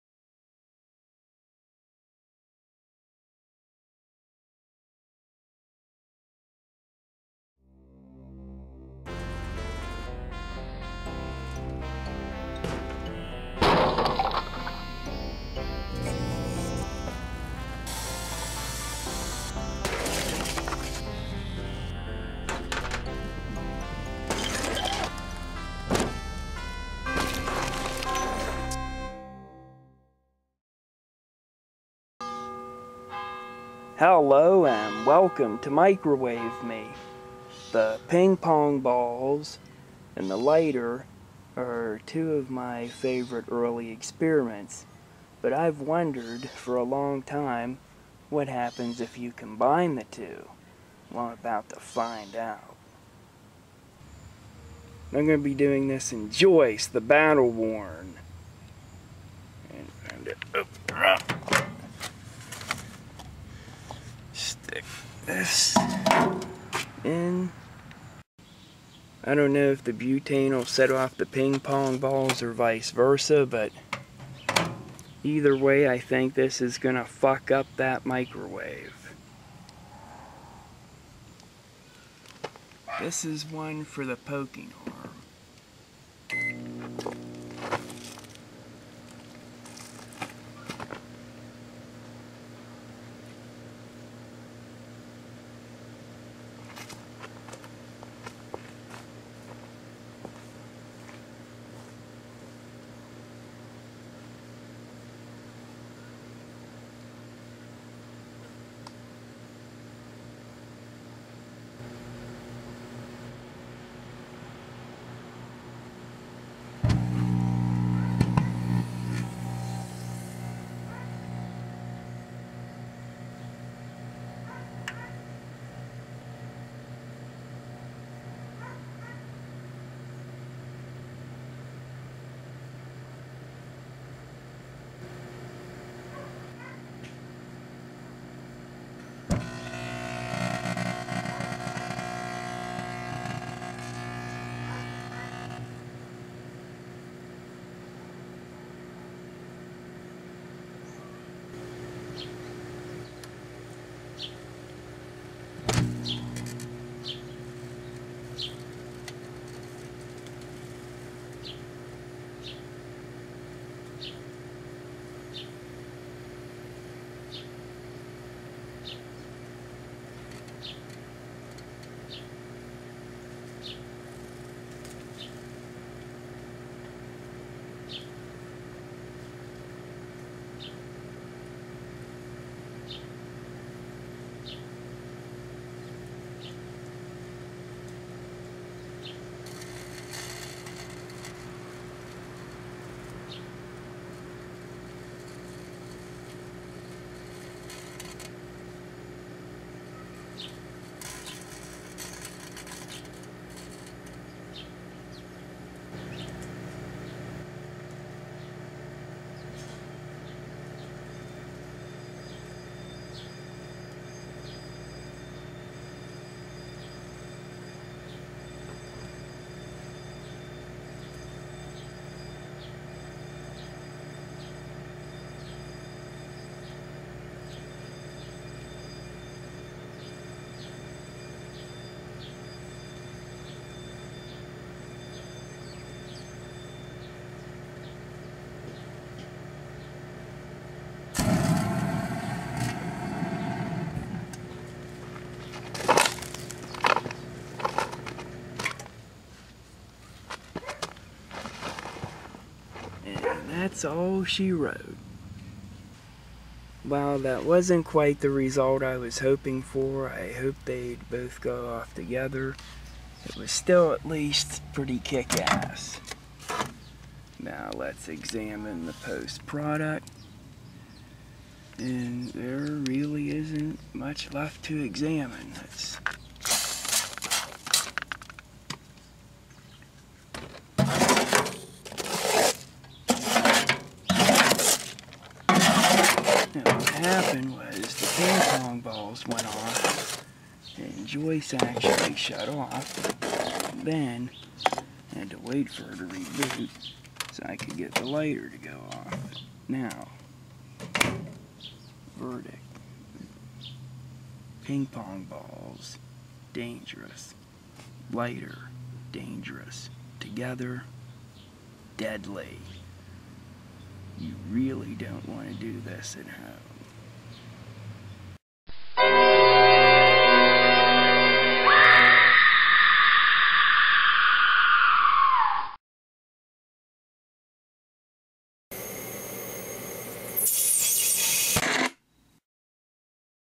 Microwave Me: Ping Pong Balls and Lighter
Experiment #347: Ping Pong Balls and Lighter

(Rated PG-13) Two of my favourite early episodes that I did were microwaving ping pong balls and microwaving a Bic lighter. Both were awesome, so I've wondered for a year and a half, what would happen if I combined the two? Would it be twice as epic? In short, no. I thought they'd both go off at the same time, one would set the other off, or something of that nature. Instead what happened was that the ping pong balls went off first, then the lighter, which seems to have melted from the ping pong balls, but it never quite went off all the way, which is peculiar because every lighter I've done has exploded or melted. This one just shot out intermittent fire. Both are dangerous ideas (formerly "really bad idea"), but combined they got a "deadly" verdict. After this and a few other experiments, Joyce needs a good cleaning, as this is the quickest a microwave ever got this dirty, surprising really. Season 11 of Microwave Me is filmed with a high definition camera in 1080p for optimal viewing experience, hosted and produced by Captain Microwave who for this episode used a Rival microwave named Joyce.